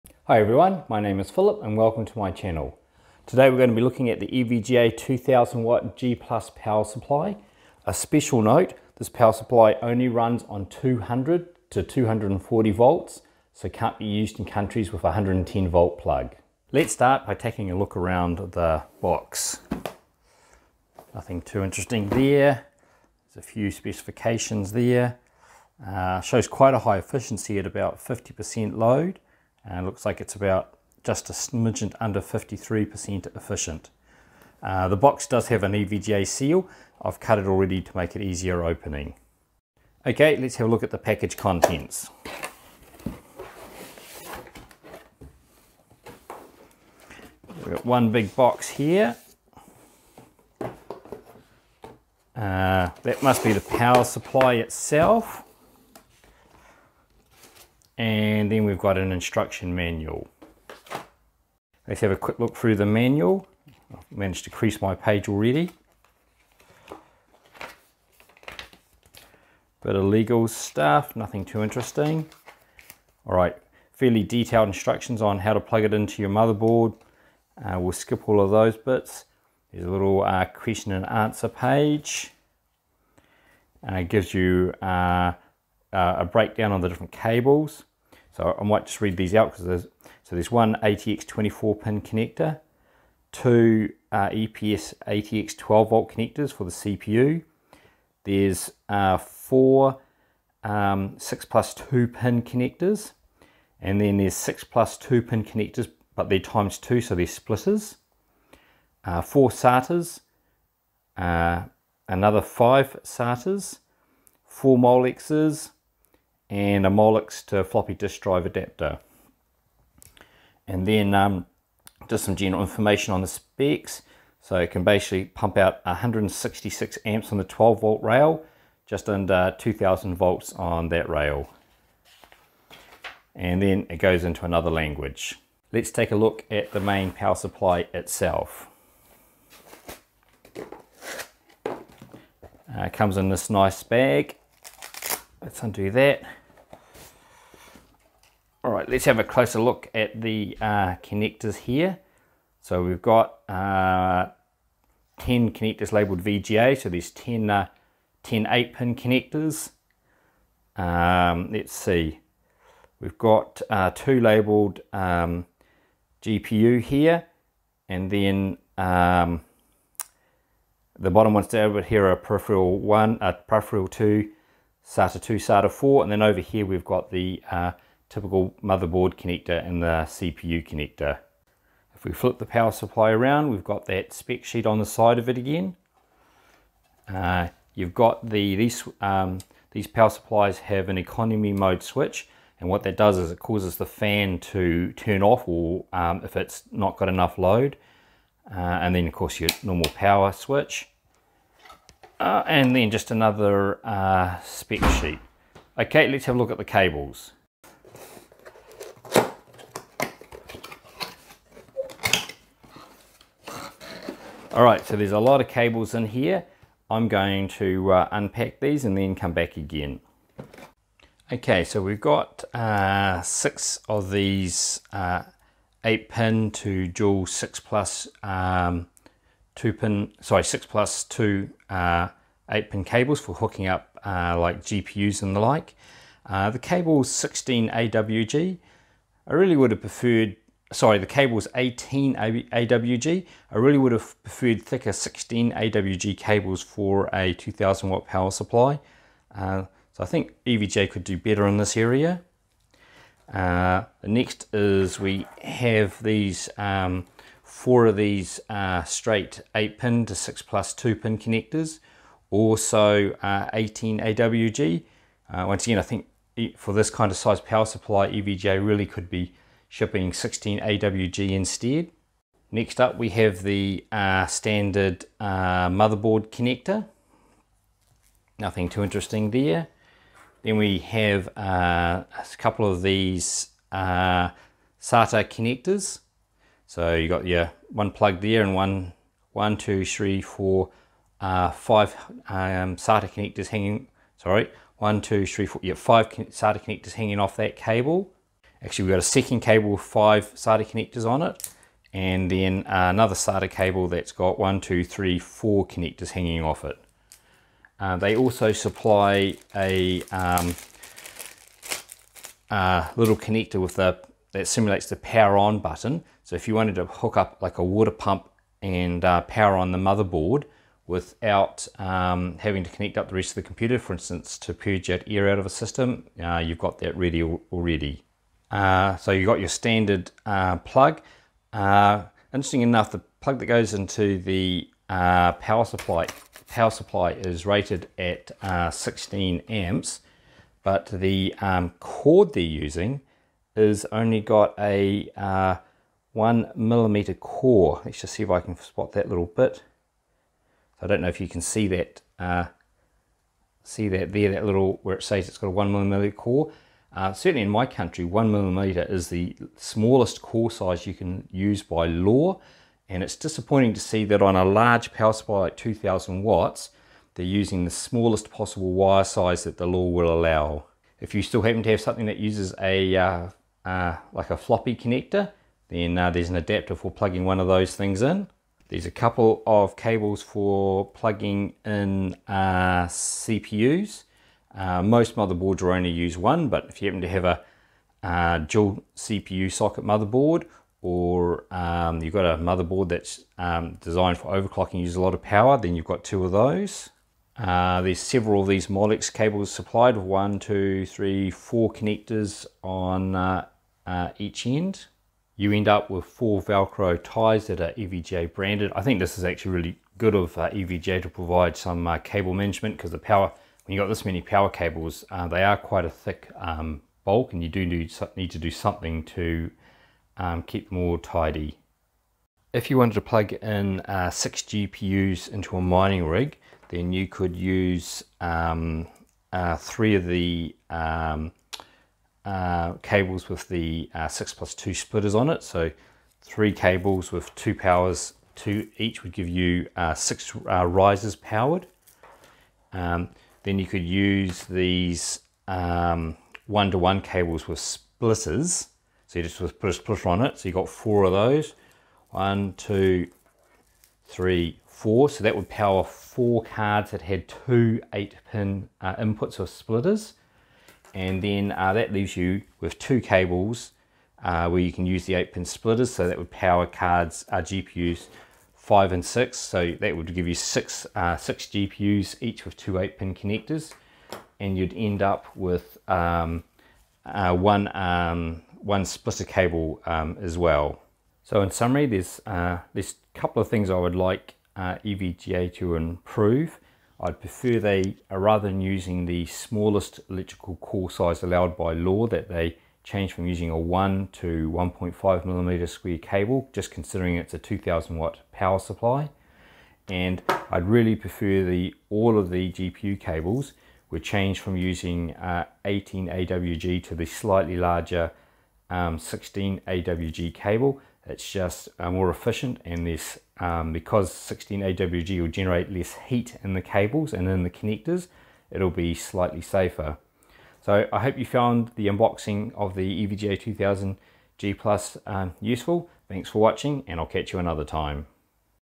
EVGA SuperNOVA 2000 G+ for Crypto Mining Unboxing
What do you get in the EVGA SuperNOVA 2000 G+ box and how many GPUs can it power in a crypto mining rig?